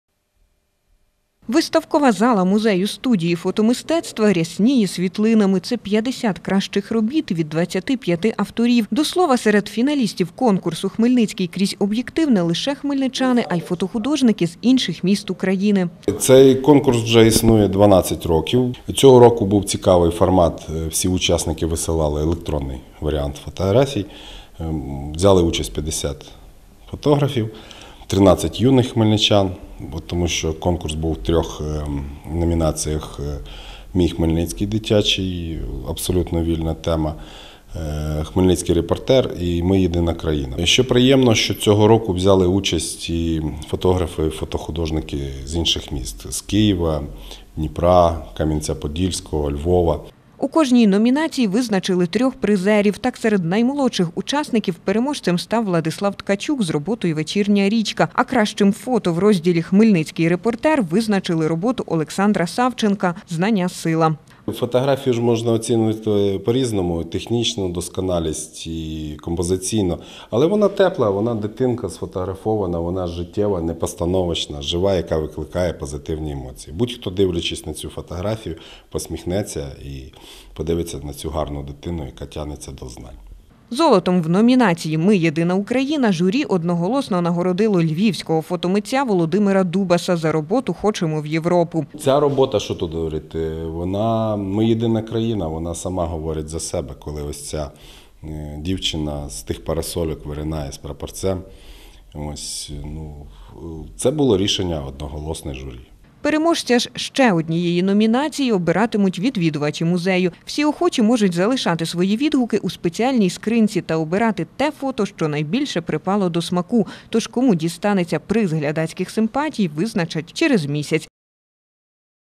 Хмельницький крізь об’єктив
"Хмельницький крізь об'єктив". У музеї-студії фотомистецтва відкрили виставку, присвячену Дню міста, та відзначили переможців конкурсу на кращу фотографію. Нагороди отримали професіонали та аматори фотосправи у таких номінаціях:  Хмельницький фоторепортер; Ми єдина країна та Мій Хмельницький. Нагадаю, цей конкурс - щорічний. Проводять його традиційно напередодні дня міста. У 2016 участь в змаганні взяли участь півсотні фотохудожників. Із 350 робіт члени журі відібрали 50 кращих світлин, і саме їх  ви зможете побачити на виставці.